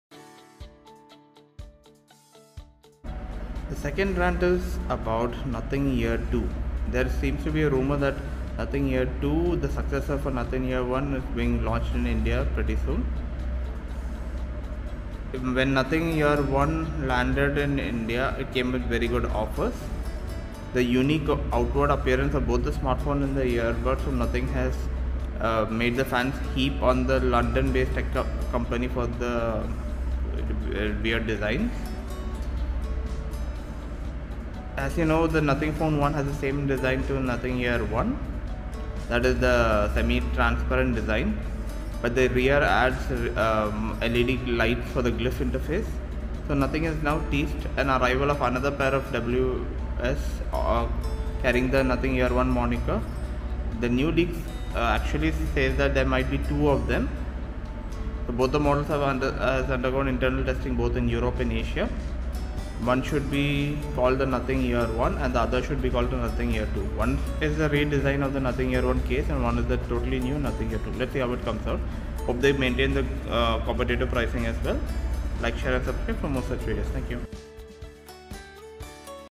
Nothing Ear(2) coming soon India nothingphone1 nothingear(1)
It looks like the Nothing Ear (2) is headed to India soon. The hotly-anticipated Nothing Ear (1) successor is reportedly in the works. Nothing took the smartphone realm by storm by launching its maiden handset dubbed the Phone (1). Notably, Phone (1) landed in India with unmissable launch offers last month. The unique outward appearance of smartphones turned heads. Nothing fans heaped praises on the London-based tech company for its ingenuity.
Interestingly, the Phone (1) design draws inspiration from Ear (1) TWS earbuds’ awe-inspiring design language. The handset retains the semi-transparent design of the earbuds. However, it adds rear-mounted LED lights for its Glyph Interface. Now, Nothing has teased the imminent arrival of another pair of TWS earbuds. The word on the street is that the purported TWS will carry the Nothing Ear (1) Stick moniker. However, a new leak suggests that the company will launch two new wireless earphones soon.[Exclusive] Nothing is working on not one, but two wireless earphones. Both models have started undergoing internal testing in various European and Asian regions.
While one could be the Nothing Ear (1) Stick which I leaked earlier, the other could be the Ear (2)